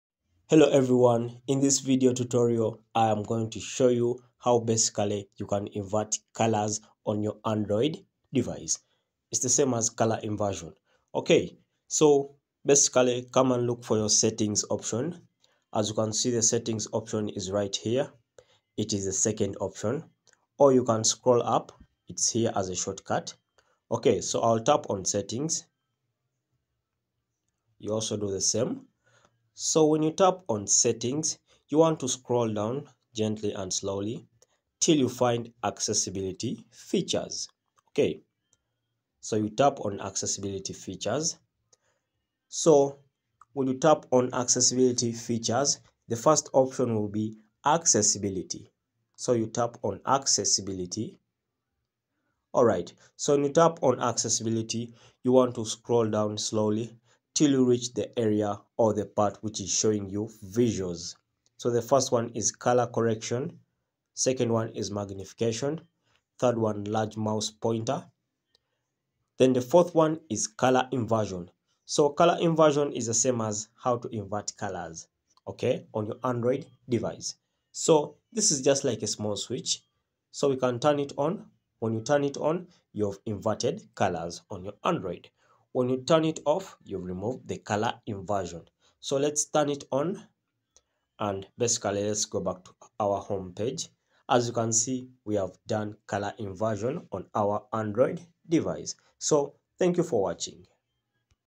How To Invert Colors On Android Phone (2023)
android Learn how to invert colors on Android phone easily.